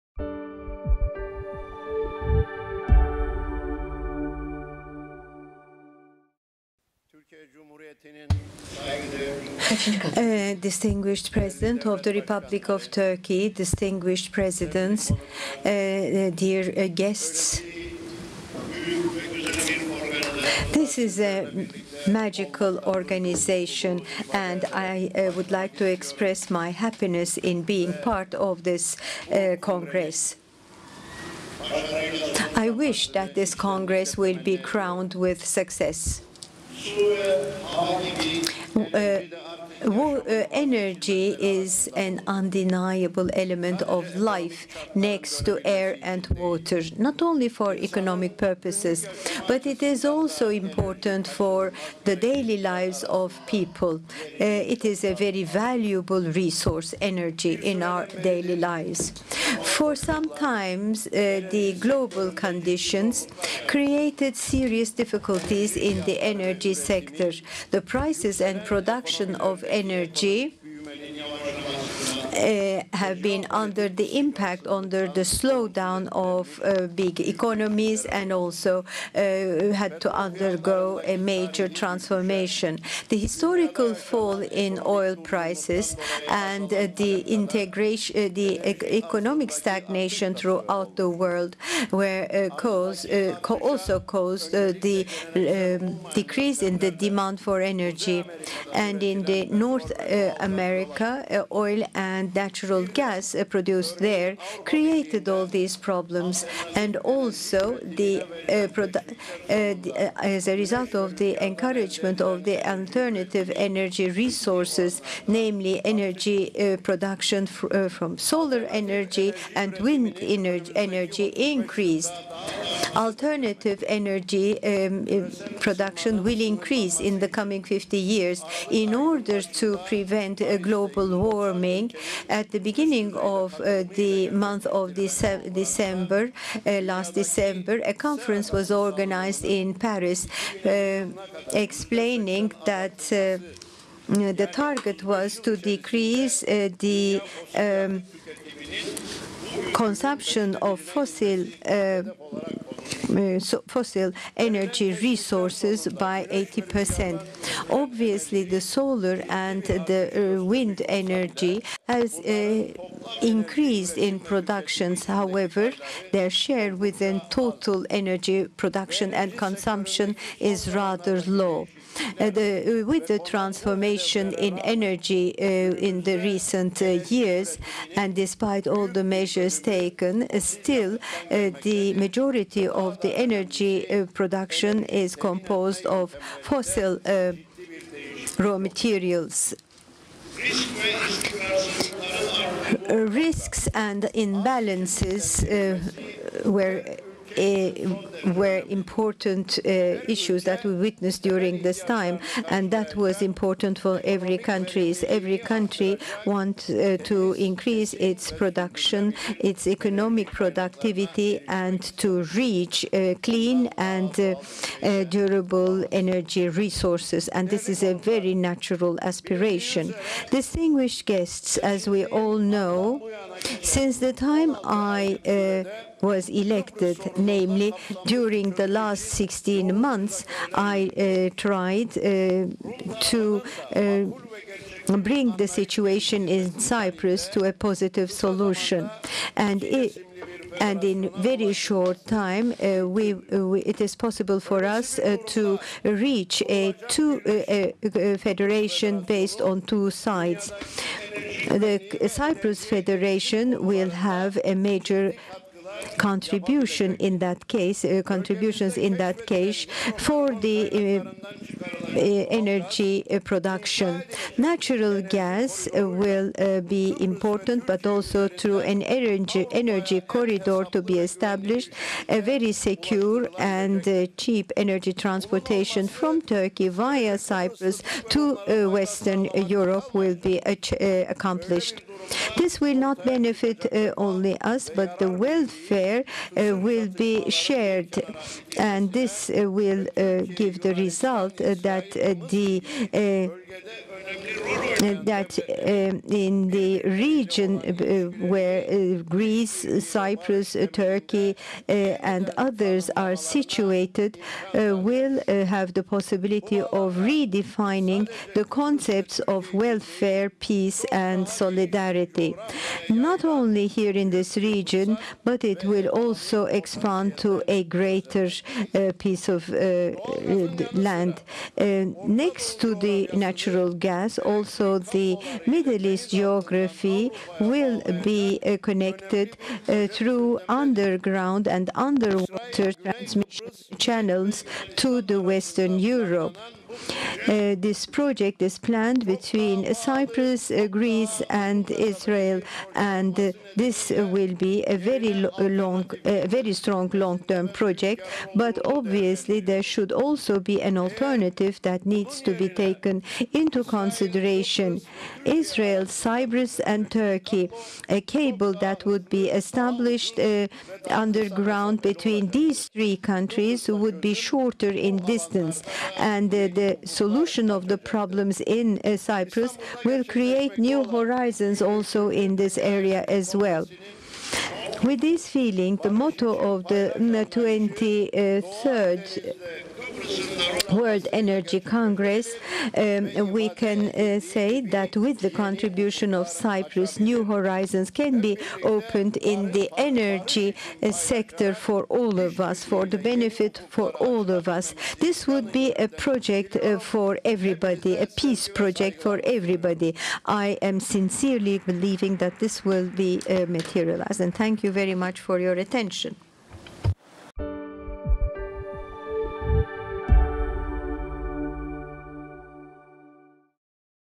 World Energy Congress | Mustafa Akıncı, President, Republic of Northern Cyprus
Keynote speech from Mustafa Akıncı, President, Government of the Turkish Republic of Northern Cyprus